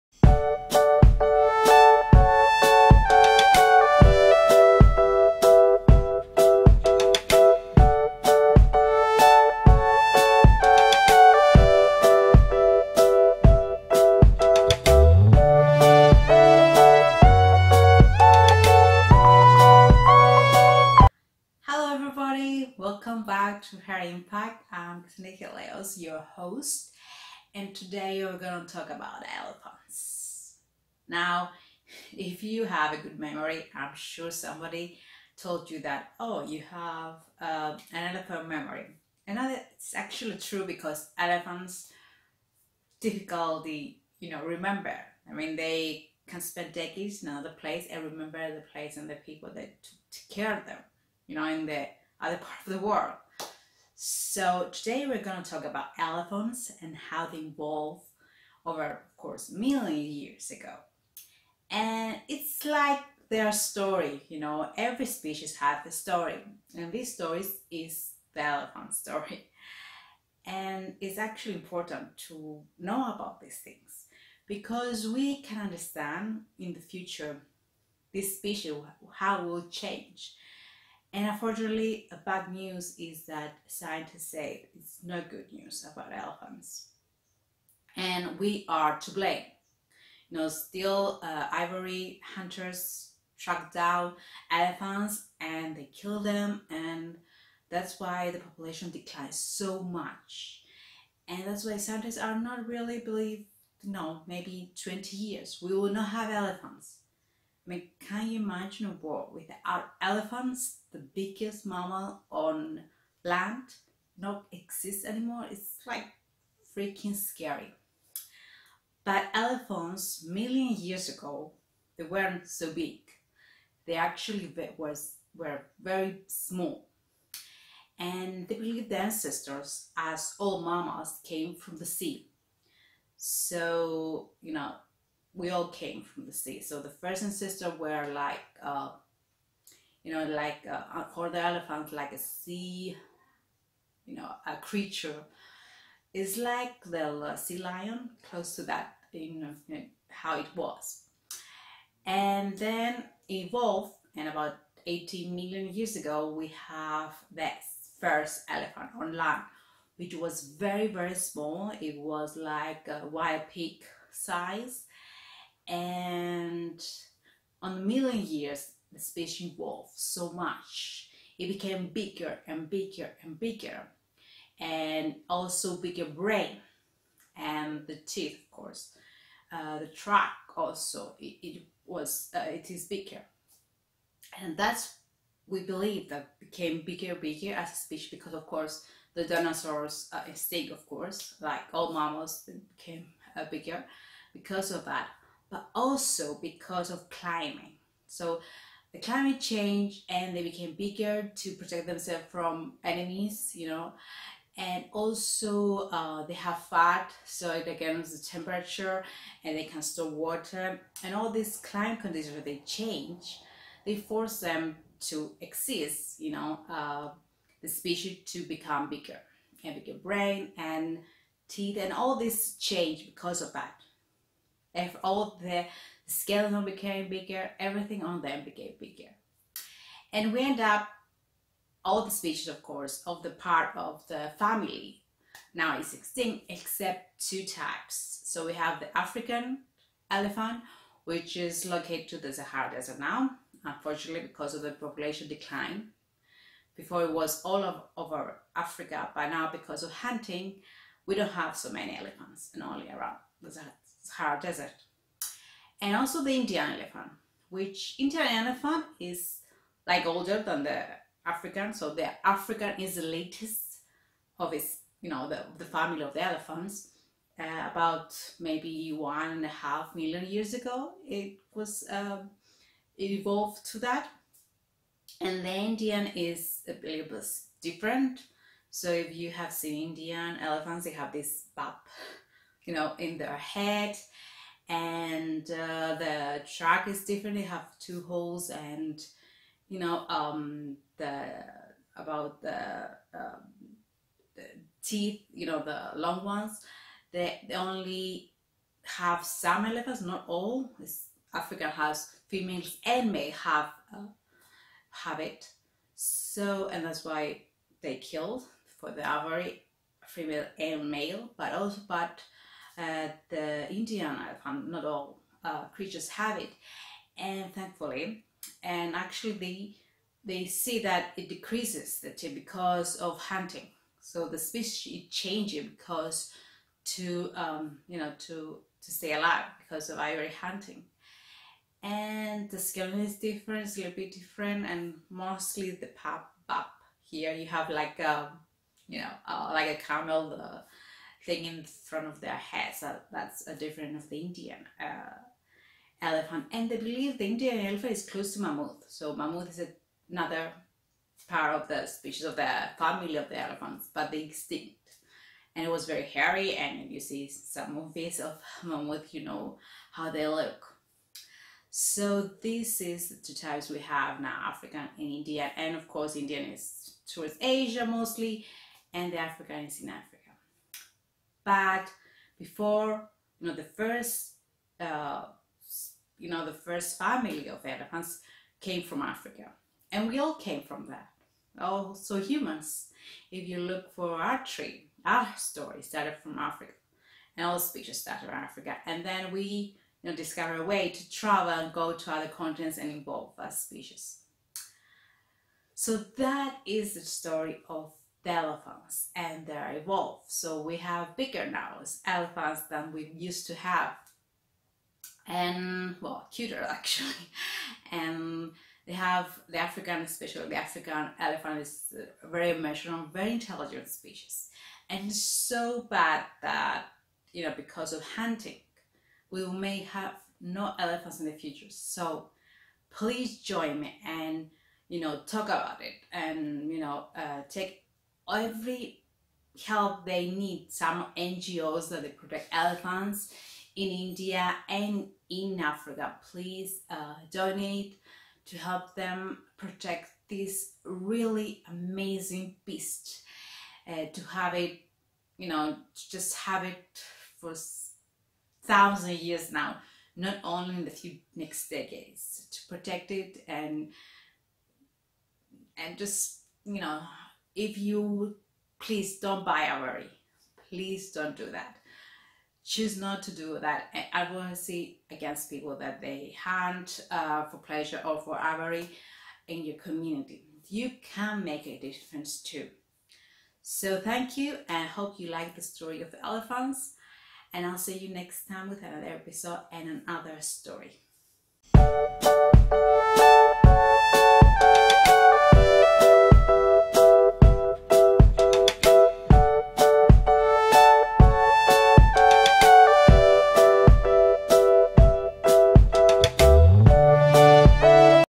Elephant evolution: How elephants changed over time on earth
The evolution story of the elephants going back million years: how their ancestors looked like.

Any questions on what you learn?